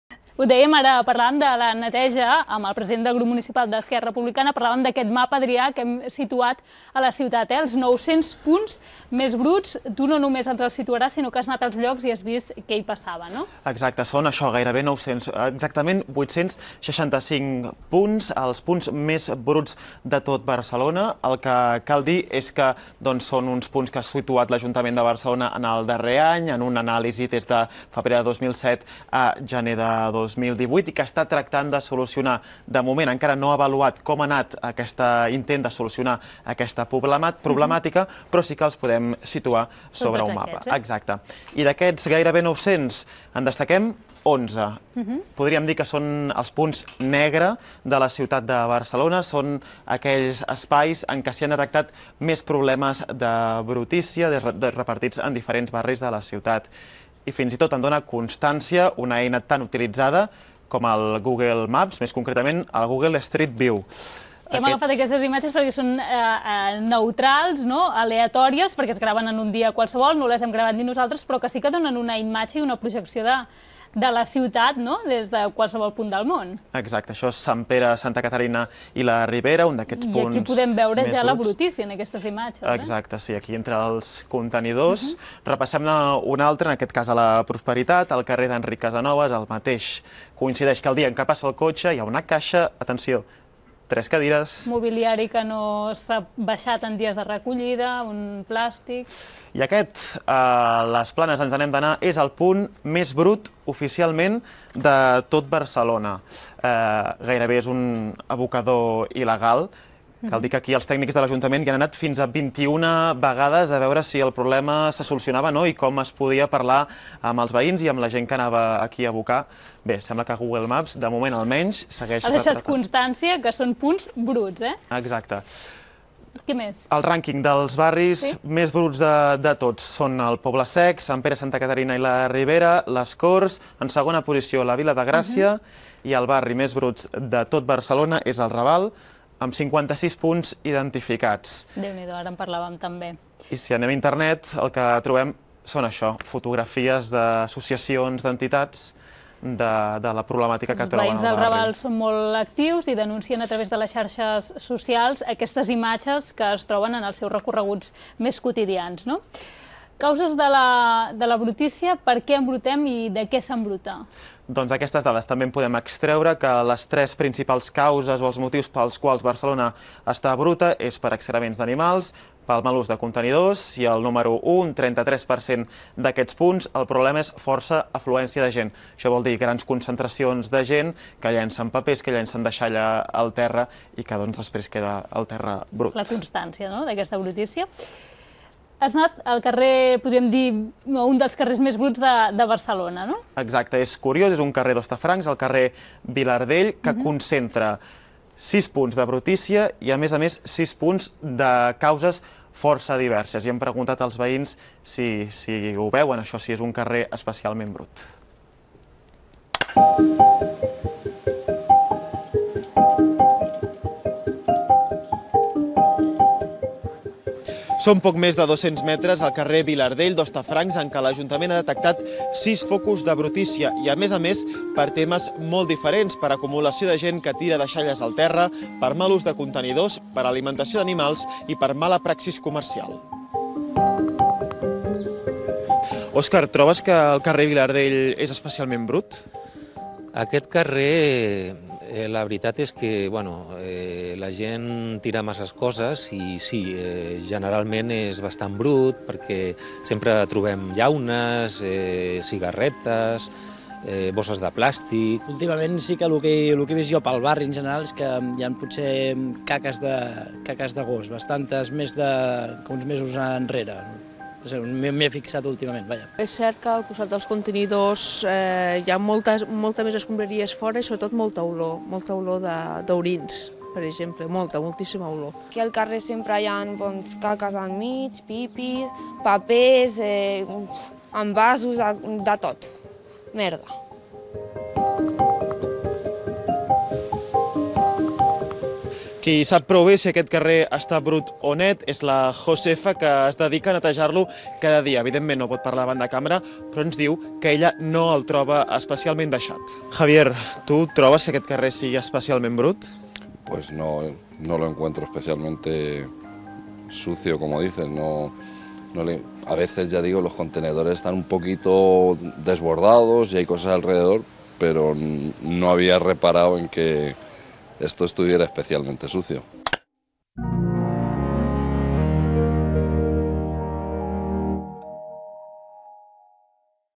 Brutícia a Barcelona - Bàsics | betevé
L’Ajuntament detecta i analitza els punts més crítics de neteja a la ciutat.

A Barcelona hi ha 865 punts amb problemes persistents de neteja. L'Ajuntament els ha avaluat durant un any amb observadors al carrer. Es tracta de racons crítics on, per exemple, es fa un mal ús dels contenidors i les papereres, hi ha propietaris incívics d'animals o mala praxi comercial. Del total de punts bruts, n'hi ha 11 que destaquen per haver obtingut una nota pitjor. Navegueu pel mapa per conèixer el motiu de brutícia de cada indret i com es distribueixen per barris. El mapa té un selector amb dues capes, una mostra els resultats per nivell de brutícia i l'altra, per la causa d'aquesta brutícia.